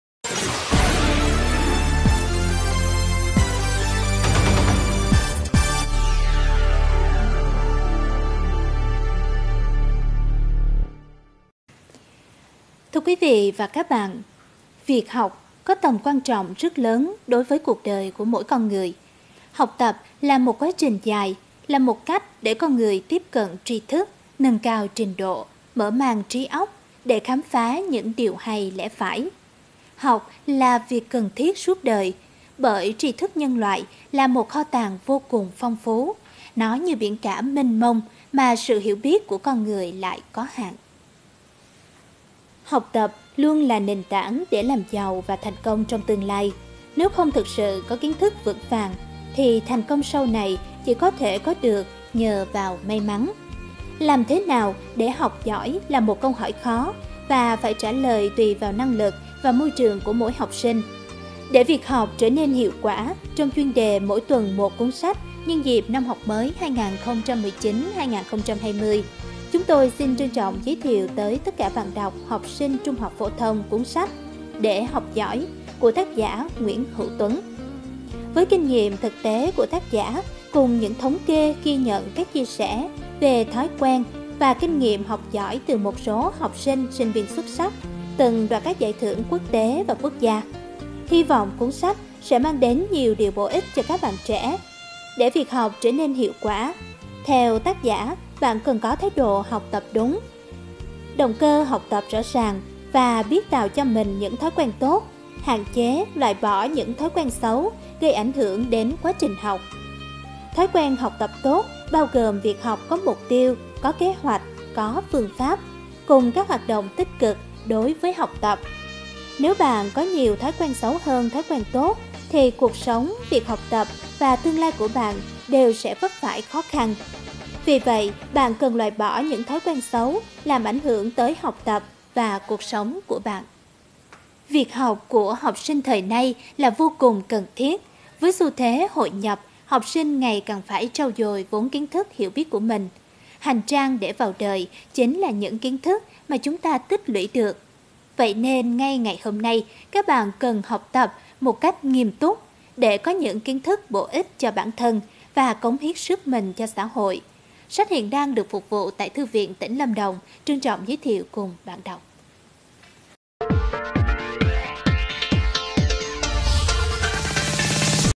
Giới thiệu sách "Để học giỏi "
Thư viện tỉnh Lâm Đồng giới thiệu sách "Để học giỏi" trên đài Phát thanh Truyền hình tỉnh Lâm Đồng ngày 09/09/2019 trong chuyên mục Câu lạc bộ Văn nghệ